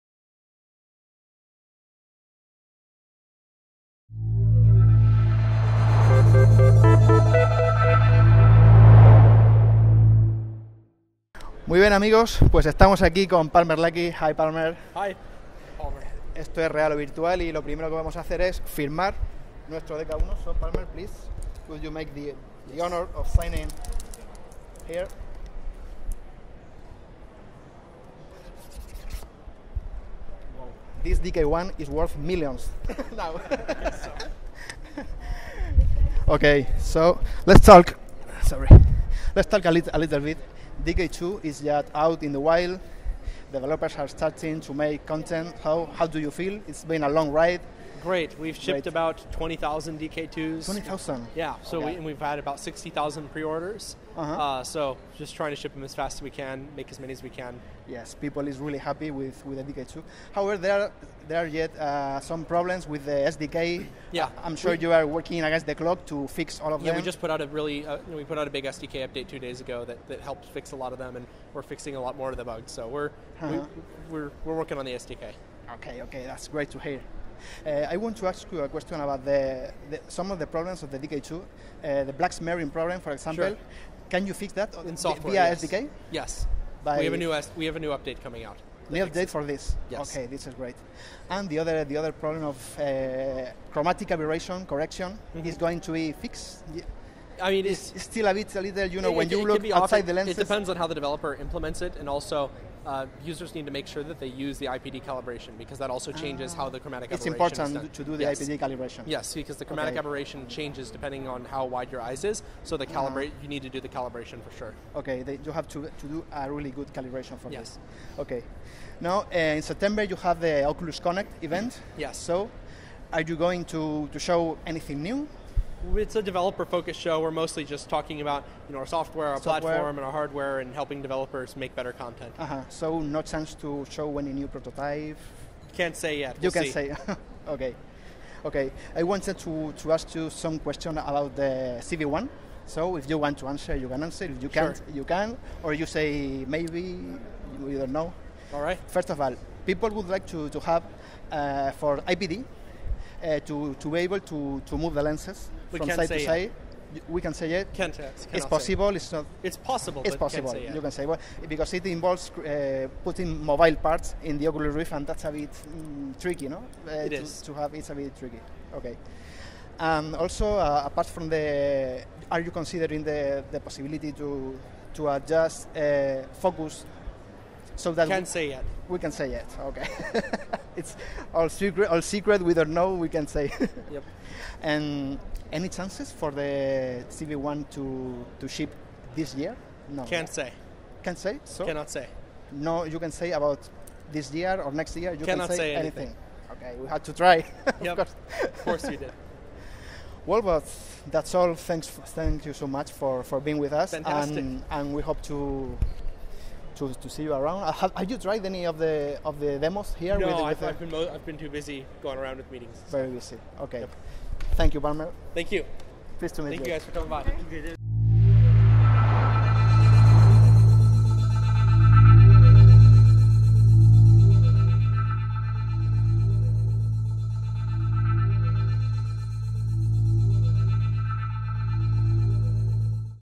Palmer Luckey - Oculus: ENTREVISTA
A pesar de lo precipitado y poco profesional de la entrevista, y de que no nos salió tan bien como nos hubiera gustado, aquí tenéis nuestra breve charla con Palmer Luckey en la Gamescom 2014. Os prometemos una entrevista en condiciones para la próxima ocasión.

No olvidéis activar los subtítulos en español.